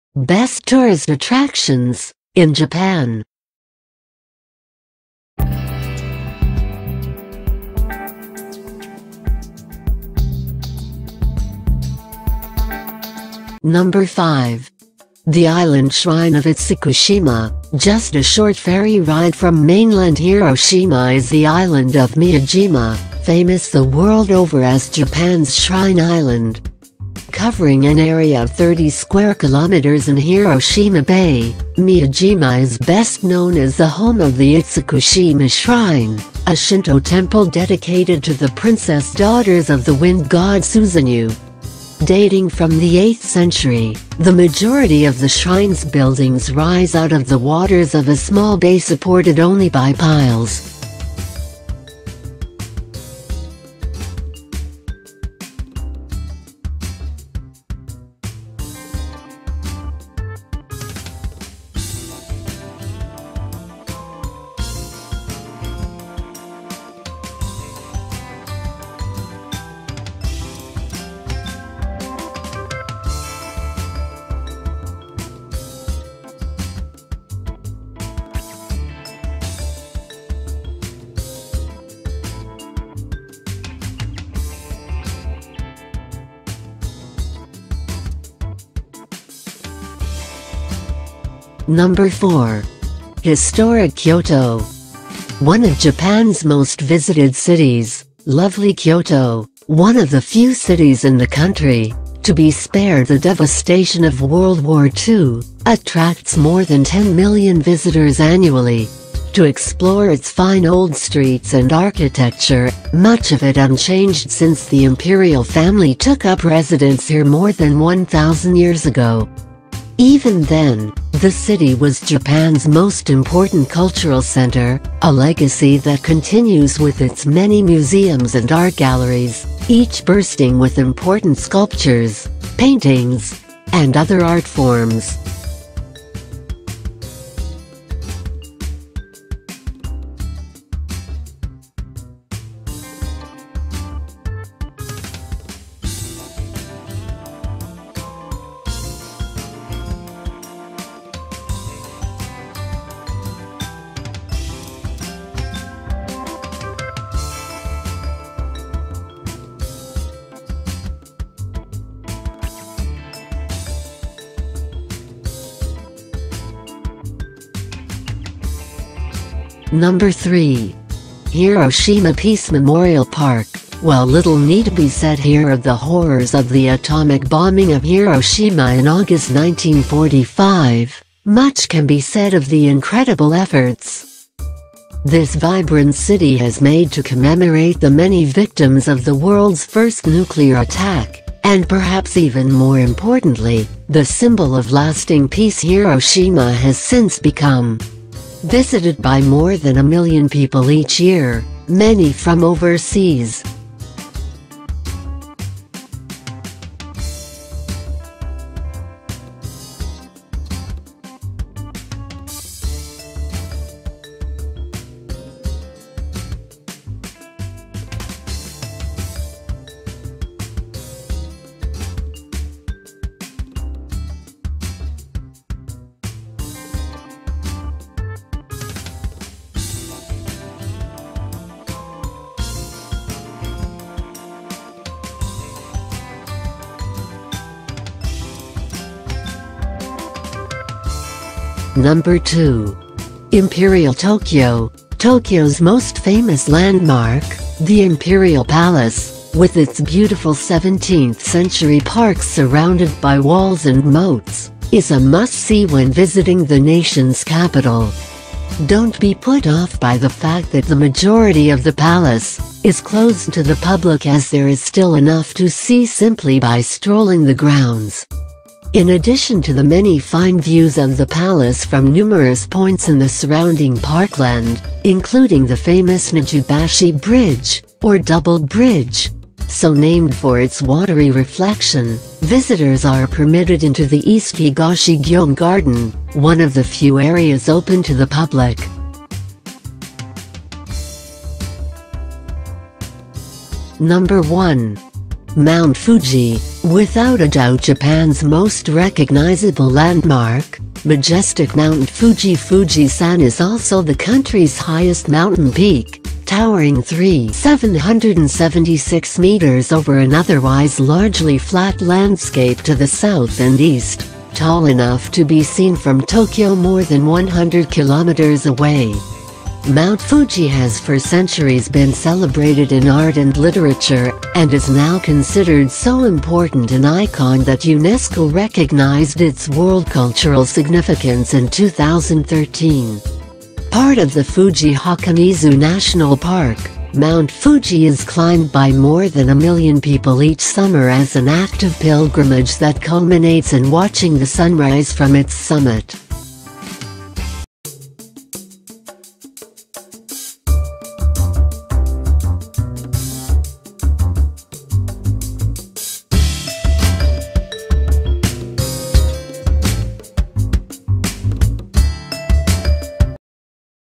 Best tourist attractions in japan
1. Mount Fuji: Without a doubt Japan's most recognizable landmark, majestic Mount Fuji (Fuji-san) is also the country's highest mountain peak, towering 3,776 meters over an otherwise largely flat landscape to the south and east, tall enough to be seen from Tokyo more than 100 kilometers away. Mount Fuji has for centuries been celebrated in art and literature, and is now considered so important an icon that UNESCO recognized its world cultural significance in 2013. Part of the Fuji-Hakone-Izu National Park, Mount Fuji is climbed by more than a million people each summer as an act of pilgrimage that culminates in watching the sunrise from its summit.

2. Imperial Tokyo: Tokyo's most famous landmark, the Imperial Palace, with its beautiful 17th-century parks surrounded by walls and moats, is a must-see when visiting the nation's capital. Don't be put off by the fact that the majority of the palace is closed to the public (it's still in use by the Imperial family), as there is still enough to see simply by strolling the grounds. In addition to the many fine views of the palace from numerous points in the surrounding parkland - including the famous Nijubashi Bridge, or "double bridge," so named for its watery reflection - visitors are permitted into the East Higashi-Gyoen Garden, one of the few areas open to the public.

3. Hiroshima Peace Memorial Park: While little need be said here of the horrors of the atomic bombing of Hiroshima in August 1945, much can be said of the incredible efforts this vibrant city has made to commemorate the many victims of the world's first nuclear attack, and perhaps even more importantly, the symbol of lasting peace Hiroshima has since become. Visited by more than a million people each year, many from overseas, Hiroshima Peace Memorial Park (Hiroshima Heiwa Kinen Koen) lies at the epicenter of the atomic blast in what was once a bustling part of the city and includes a number of important monuments, memorials, and museums relating to the events of that fateful day.

4. Historic Kyoto: One of Japan's most visited cities, lovely Kyoto - one of the few cities in the country to be spared the devastation of WWII - attracts more than 10 million visitors annually to explore its fine old streets and architecture, much of it unchanged since the Imperial family took up residence here more than 1,000 years ago. Even then, the city was Japan's most important cultural center, a legacy that continues with its many museums and art galleries, each bursting with important sculptures, paintings, and other art forms.

5. The Island Shrine of Itsukushima: Just a short ferry ride from mainland Hiroshima is the island of Miyajima, famous the world over as Japan's Shrine Island. Covering an area of 30 square kilometers in Hiroshima Bay, Miyajima is best known as the home of the Itsukushima Shrine, a Shinto temple dedicated to the Princess daughters of the wind god Susanoo. Dating from the eighth century, the majority of the shrine's buildings rise out of the waters of a small bay supported only by piles. The effect at high tide is simply stunning, making these structures - including the famous Great Floating Gate (O-Torii) - appear as if they're floating on water. Linked together by walkways and bridges, it's a fascinating place to explore, in particular its larger halls such as the exquisite Honden (Main Hall), the Offerings Hall (Heiden), the Prayer Hall (Haiden), and the Hall of a Thousand Mats (Senjokaku).